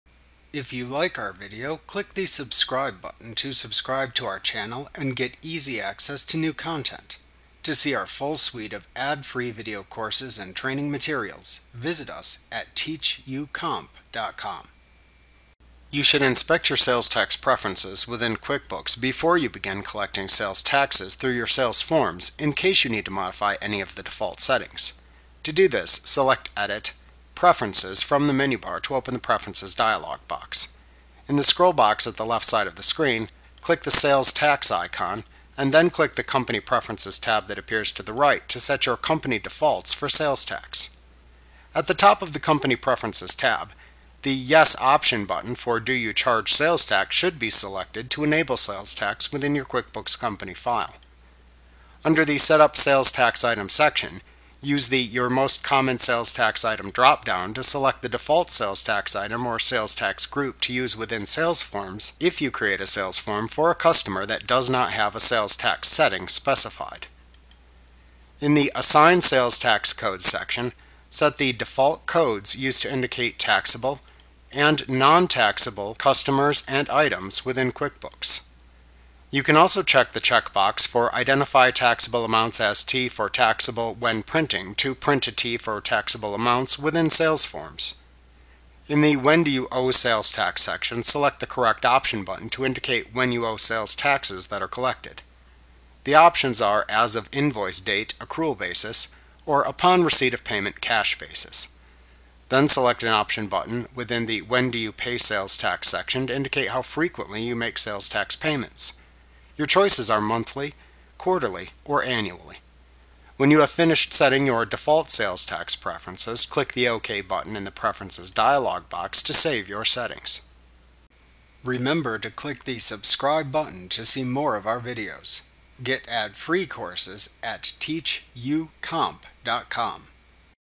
QuickBooks Desktop Pro 2021 Tutorial Setting Sales Tax Preferences Intuit Training
Learn about Setting Sales Tax Preferences in Intuit QuickBooks Pro 2021 with the complete ad-free training course